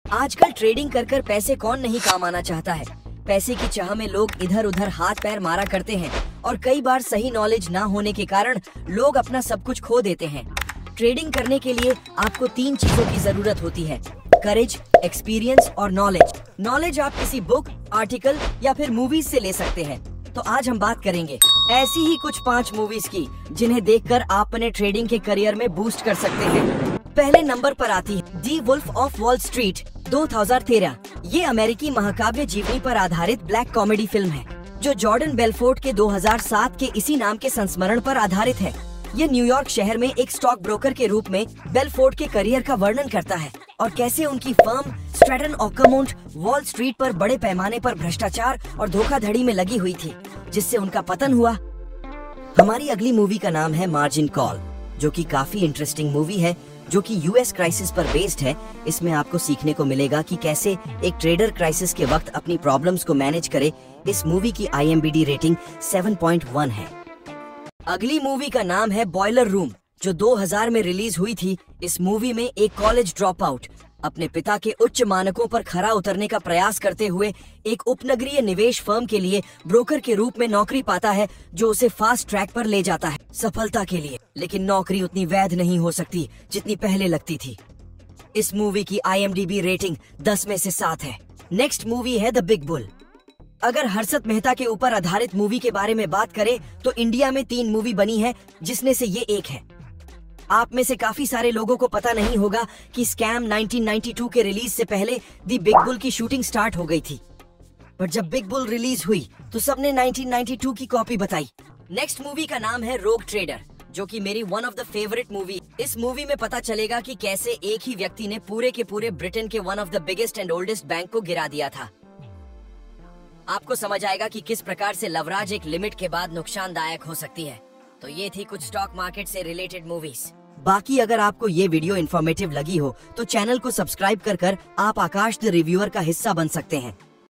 Trading Related Movies 🤑 Stock Market सीखें इन Movies से 💸
Best Trading Movies To Learn Basic Concepts of Trading, stock Market, शेर बाजार , Movies related to stock Market and Trading. To become a Trader You Need Three Thing- Courage, Experience and Knowledge . Knowledge you can acquire from any book, article and Movies.In This video I have cover Top 5 Movies related to Trading and stock market

Movie Name:-

Margin call
boiler room
the big bull/scam 1992
rouge trader

📢 Trading and stock Market से related aur Movies ke liye Comment kare Part 2 💥 Coming soon 🔜